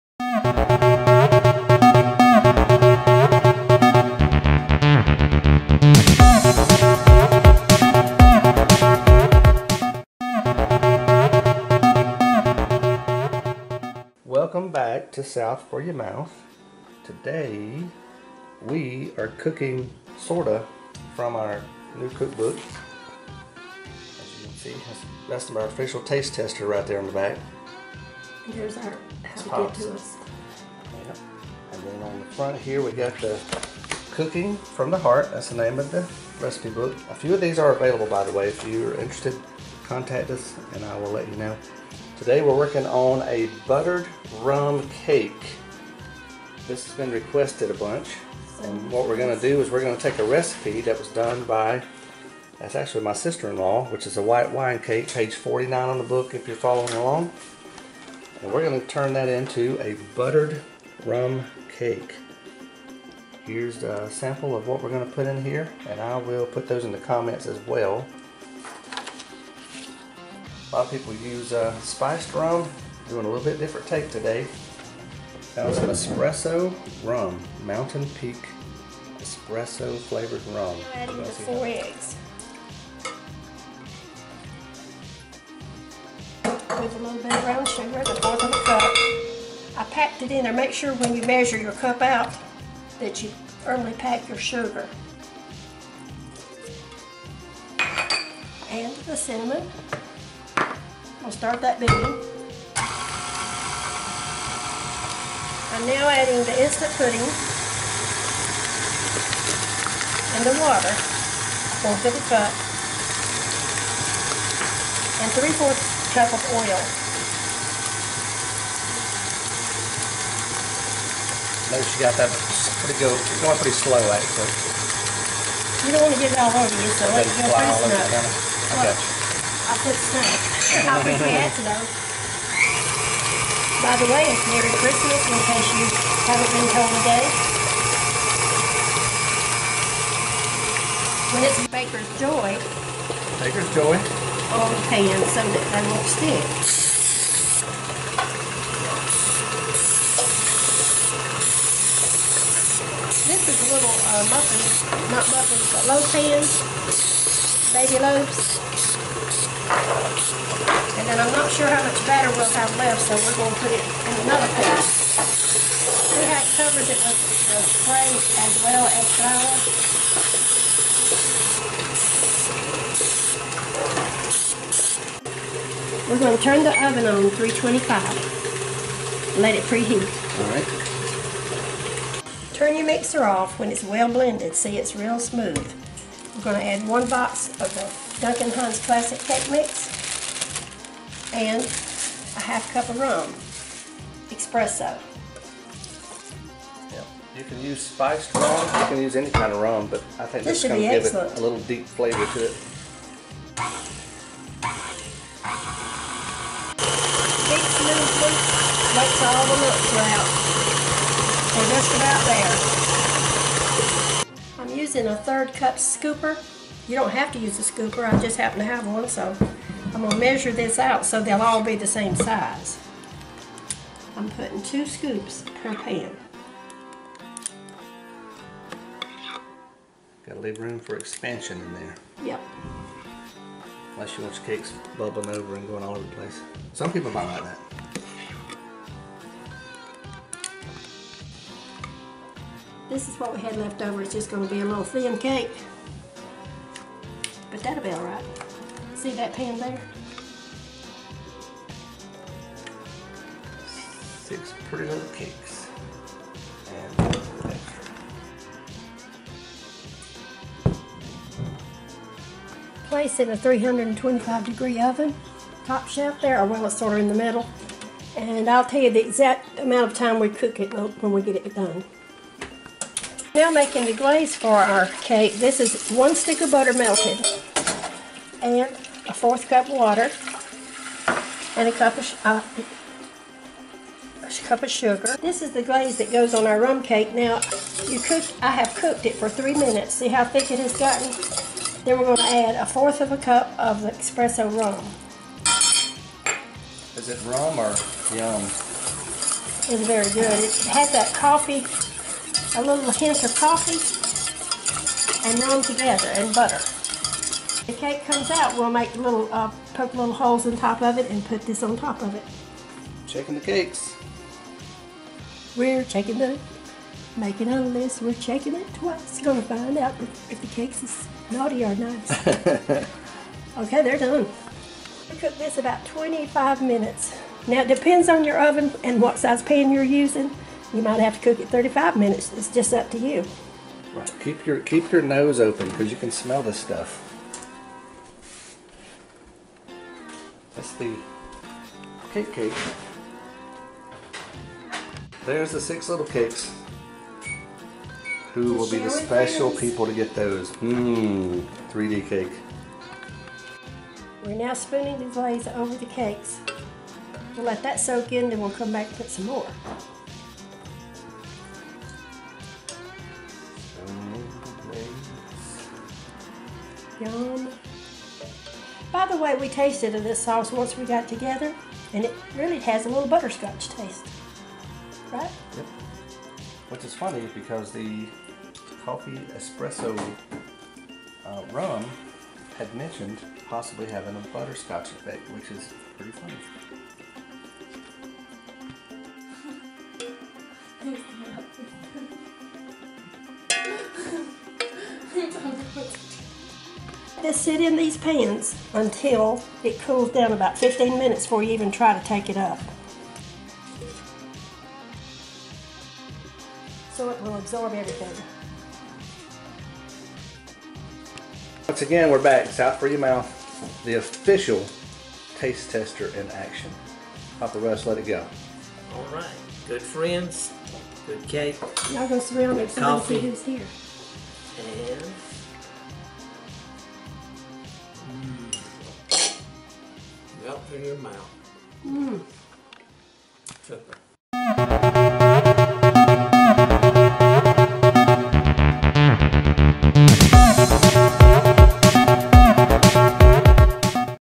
Espresso Rum Cake
Making an Espresso Rum Cake from scratch for a Holiday Treat.